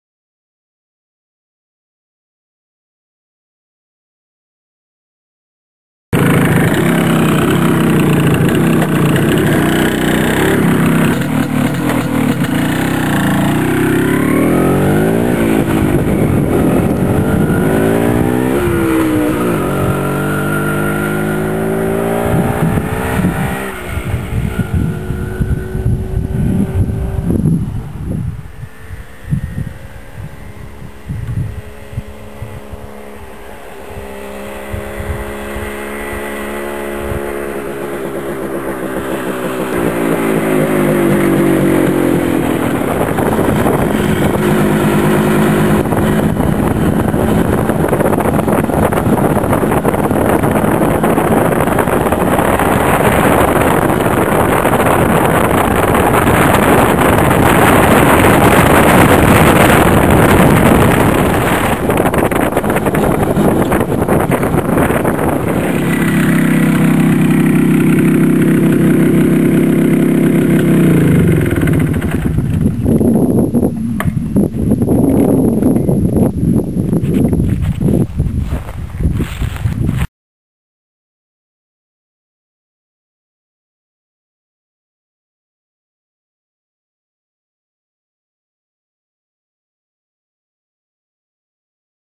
MZ ETZ 251 mix
Takie różne kawałki z moją Etką. Kawałek jazdy przez Błociszewo :-) .